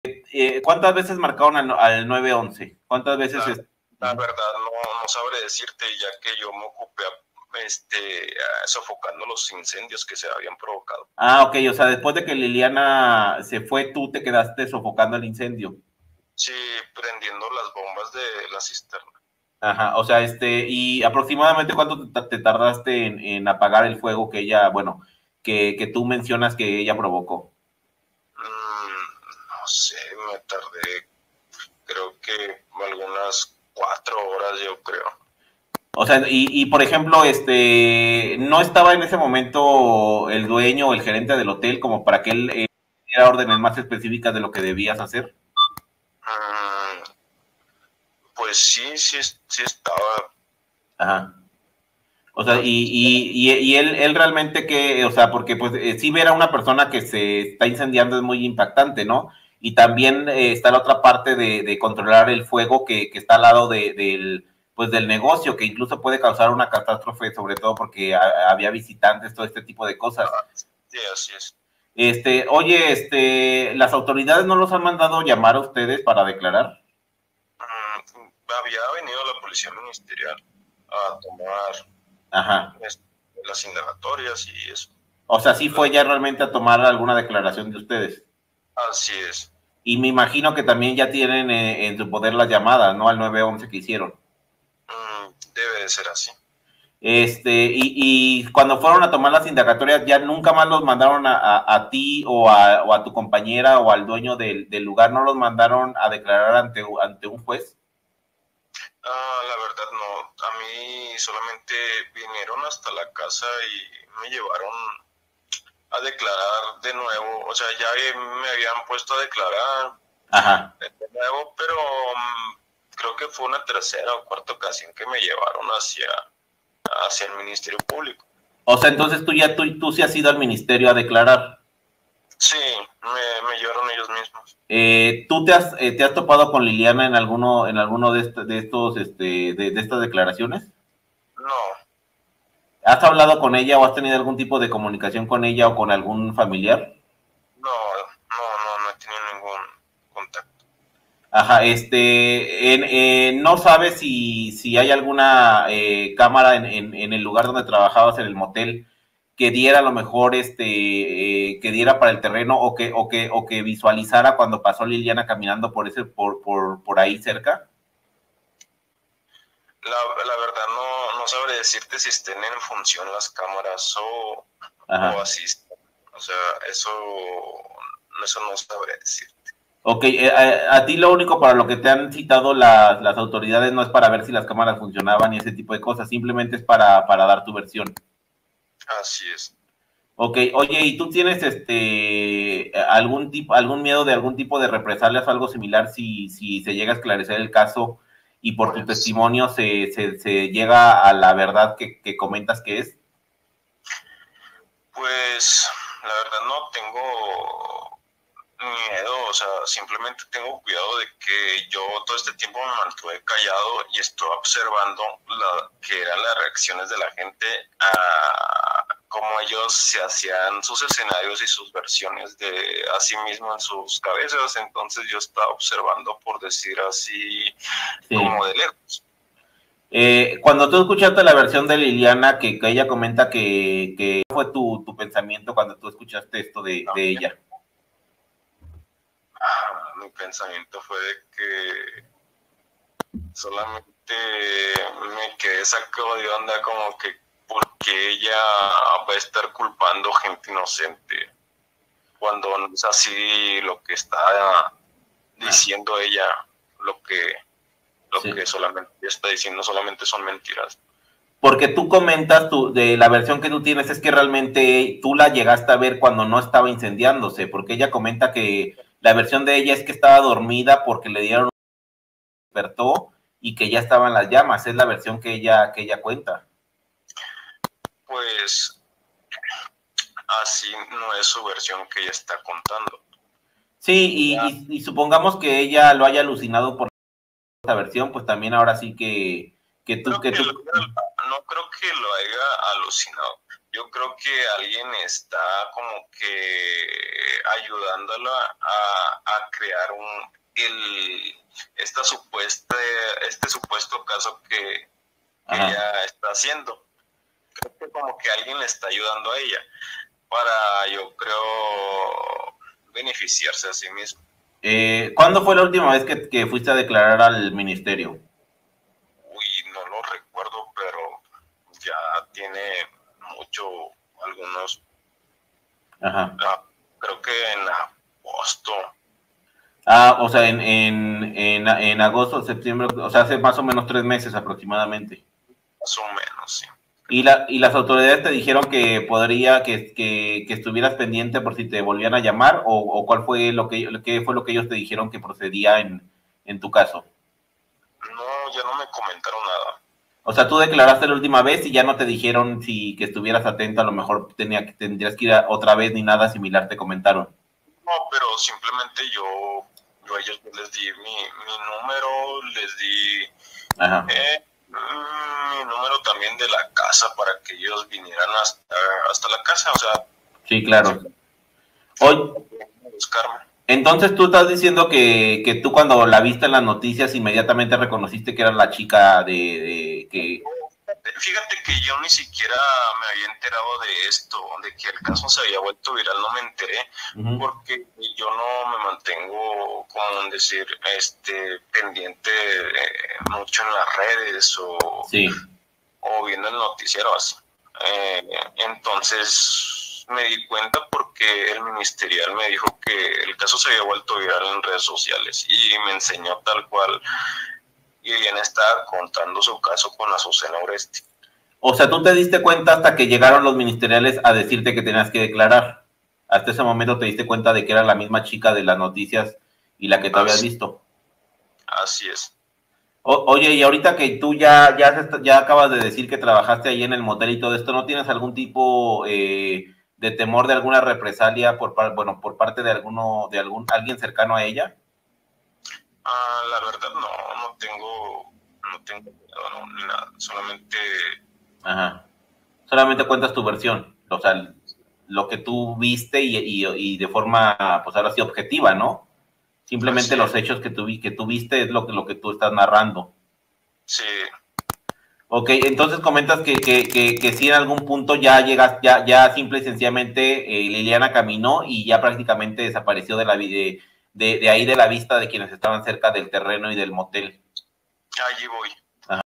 LILIANA FERNÁNDEZ QUE QUEMARON MIENTE ESTA ES LA PARTE 2 TESTIGO PRINCIPAL
PARTE 2 DE ENTREVISTA CON RUINAVERSAL DEL TESTIGO PRINCIPAL DE LOS HECHOS EN EL CASO DE LILIANA FERNANDEZ DONDE SE QUEMO POR CONSUMIR SUSTANCIAS Y MIENTE PARA SACAR DINERO